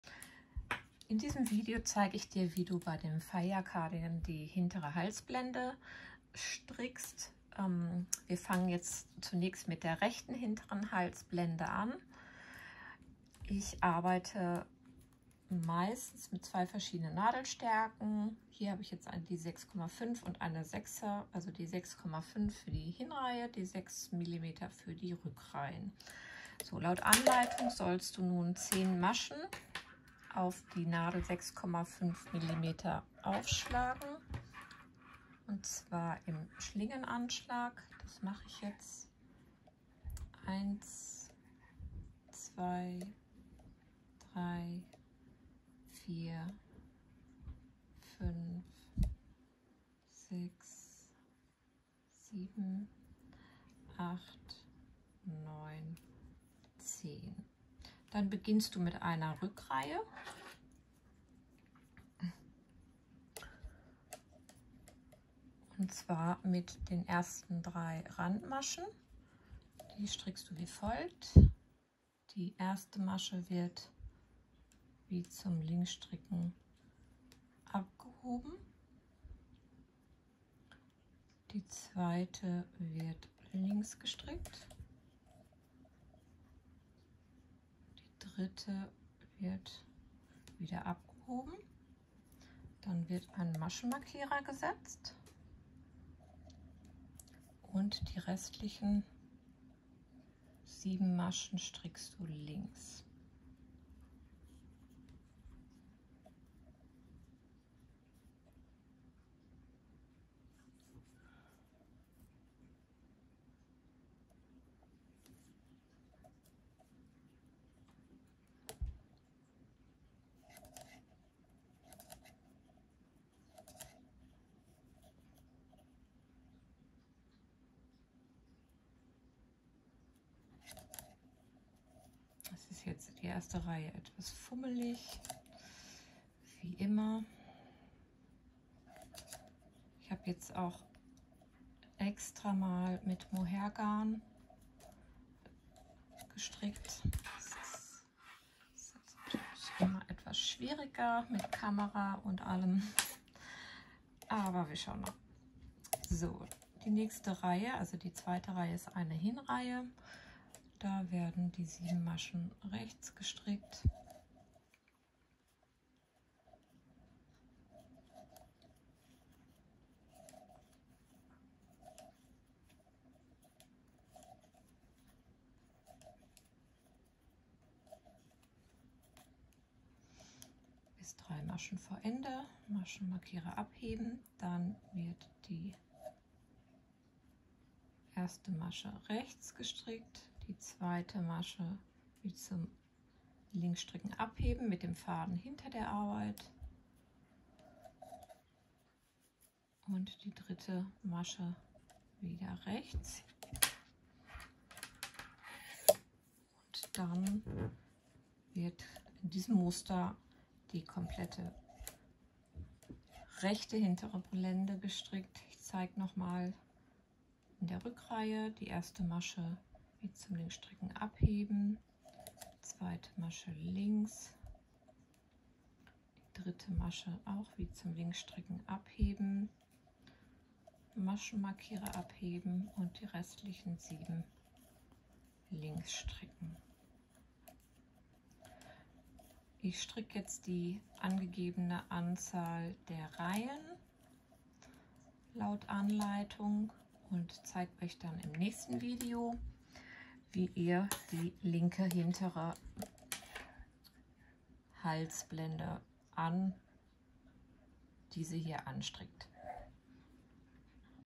FEIYA V Cardigan // Schritt 1 – Hintere Halsblende A) Rechte Halsblende
In diesem Video zeige ich Dir an einem Beispiel, wie Du die rechte hintere Halsblende beim FEIYA V Cardigan strickst.

Die genauen Maschen und Reihenangaben für Deine Größe kannst Du der Anleitung entnehmen.